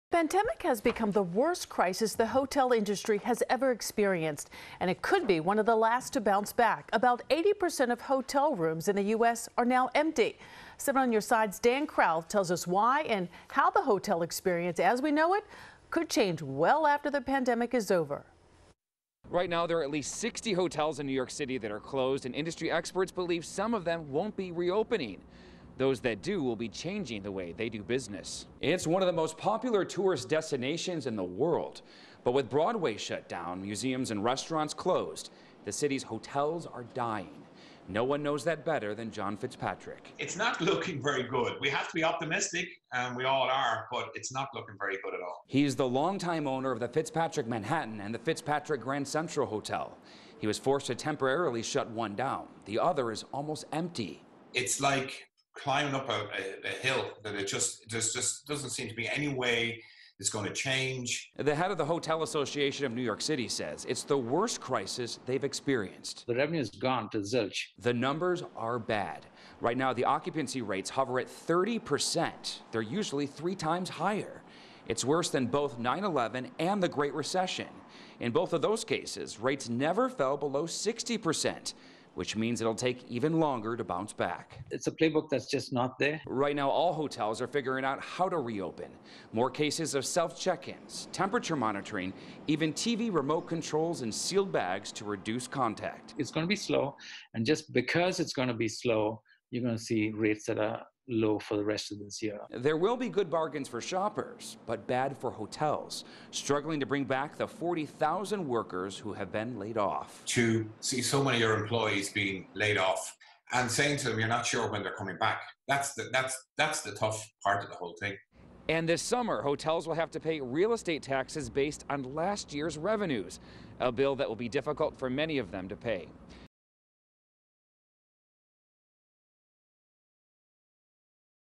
'It's worse than 9/11': Hotel industry decimated by coronavirus pandemic
John Fitzpatrick has been in the hotel business his entire life. After 9/11, he thought he faced the worst the industry could experience.

But then, the coronavirus pandemic happened.

"It's not looking very good," he said. "We have to be optimistic. We all are, but it's not looking very good at all."

He owns two hotels, the Fitzpatrick Manhattan and Fitzpatrick Grand Central Hotel. He was forced to shut down one temporarily, and the other is almost empty.

"It's just like climbing up a hill," he said. "There just doesn't seem to be any way it's going to change."

More than 60 hotels are closed in New York City due to the pandemic, and some of them aren't expected to reopen. Those that do are changing the way they do business.

NEW HERE? –

NEW HERE? –